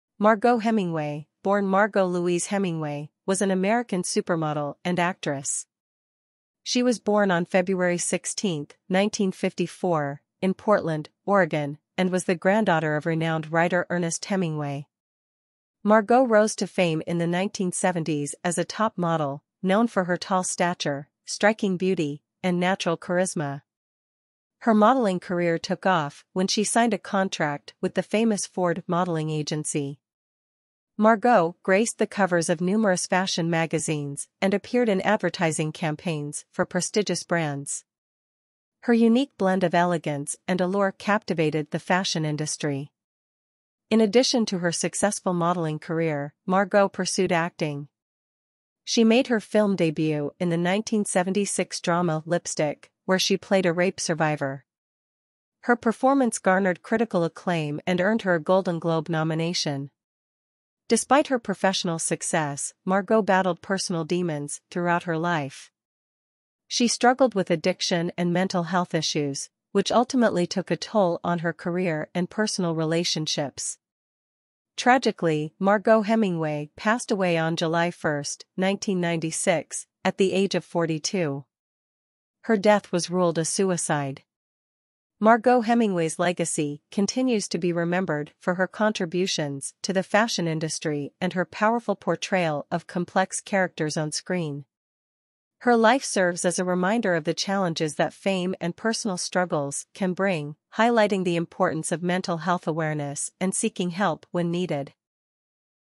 Margaux Hemingway (1954-1996)
Margaux Hemingway was an American supermodel and actress known for her striking beauty and troubled personal life. She gained fame in the 1970s and appeared in movies like "Lipstick" and "Killer Fish." Sadly, she battled addiction and tragically died at the age of 41.